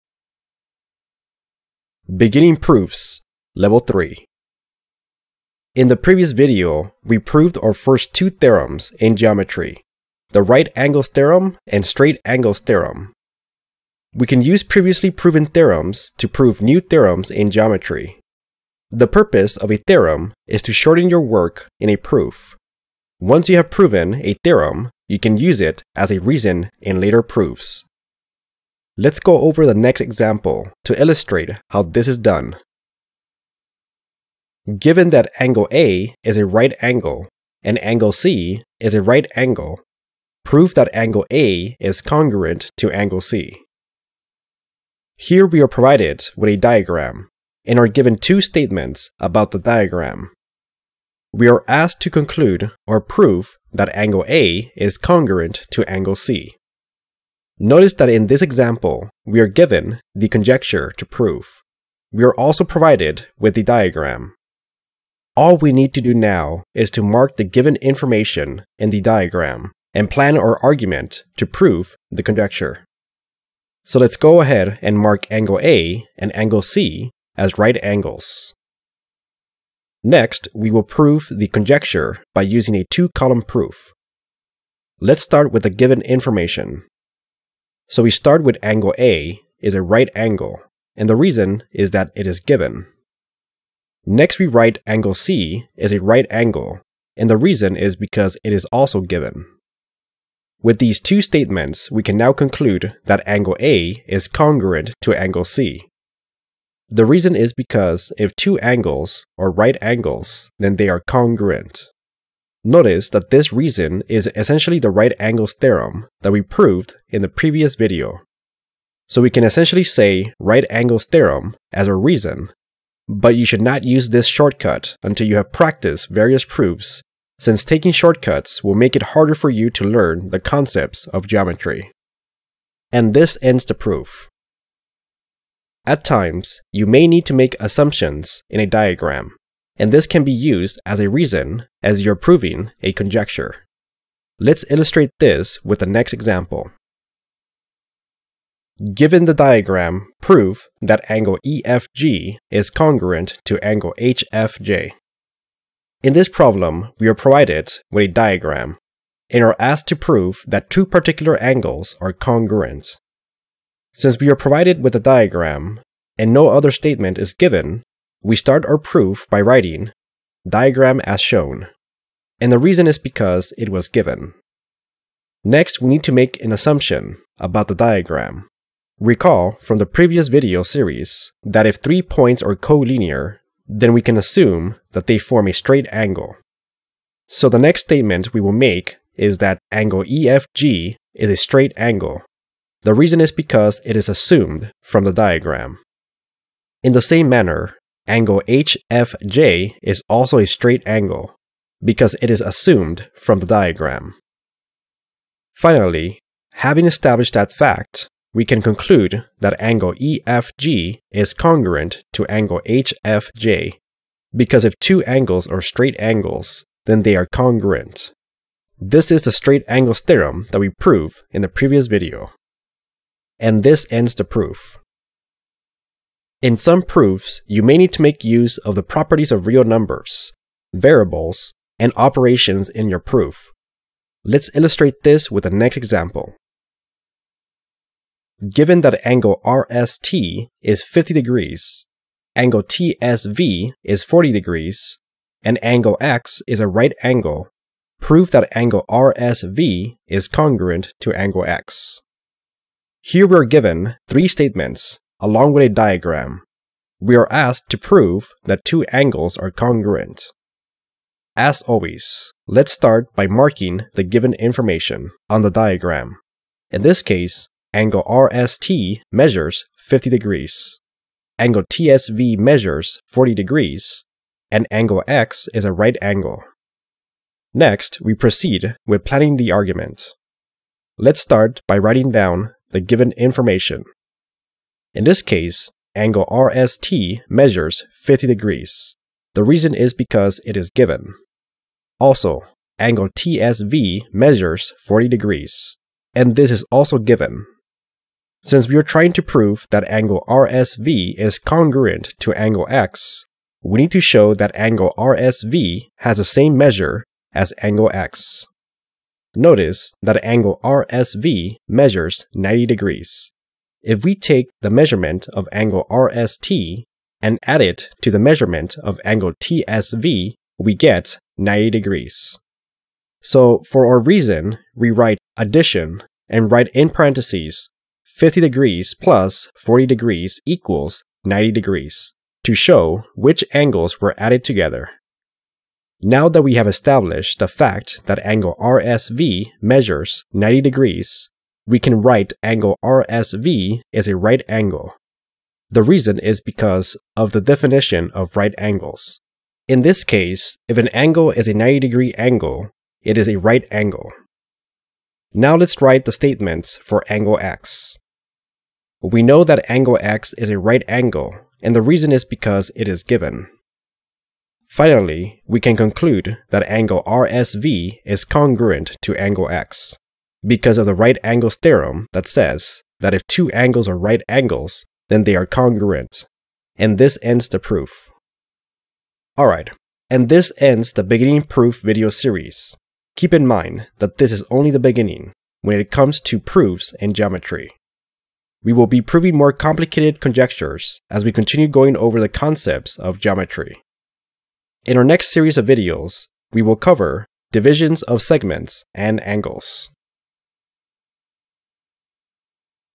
Geometry: Beginning Proofs (Level 3 of 3) | Examples
In this video we go over 3 examples that illustrates how to use previously proven theorems, assumptions, properties of real numbers, variables, and operations as a reason in a proof.

0:00 Introduction
0:32 Example 1: Proof Angles are Congruent
2:26 Example 2: Proof Angles are Congruent
3:56 Example 3: Proof Angles are Congruent